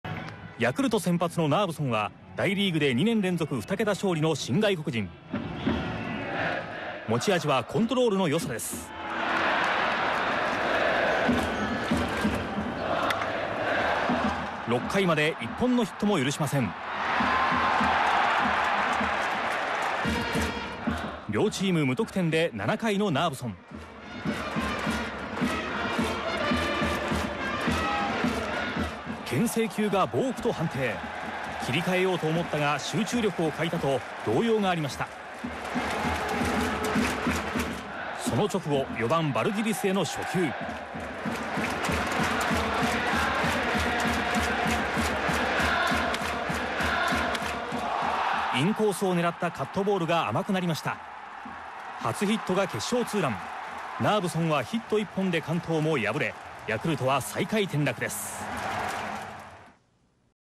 DeNA × ヤクルト 1安打勝利 2014/5/10 得点ハイライト
「ＤｅＮＡ２‐１ヤクルト」（１０日、横浜）

　文字通り、ひと振りで試合は決まった。七回１死、４番に抜てきしたバルディリスがチーム初安打となる決勝２ラン。その１安打だけでつかんだ勝利。ＤｅＮＡの中畑監督は「１安打で勝っちゃっていいのかな。これが野球だよね」と興奮していた。

　悪夢がよぎった。昨年は中日・山井に、一昨年は広島・前田に無安打無得点試合をやられた。「毎年１回経験しているから、ボチボチ？というのがありました。ドキドキだったよ」と振り返った。

　地獄から天国に導いたのがバルディリスだった。好投のモスコーソに「ホームランを打ってくる」と約束。打った瞬間、誇らしげにモスコーソを指さした。「自分のひと振りで決まって満足だよ」と予告弾を喜んだ。

　中畑監督は「あの１発で血管が３本くらい切れたよ」とニヤリ。ブランコが故障。中村が２軍落ちで４番不在だったが「当分、これでいいんじゃない？」と新主砲に指名。これでヤクルトを抜いて５位、４位中日も１ゲーム差。横浜スタジアム１０００勝を劇的に飾った。